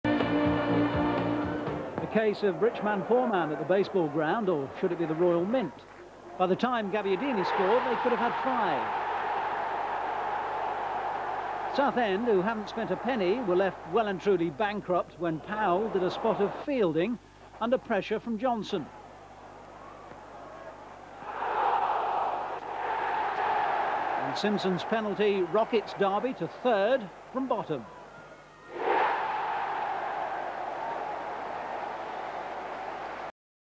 1992-93 Derby County 2 Southend Utd 0 - 26/09/1992
Derby goals Marco Gabbiadini , Paul Simpson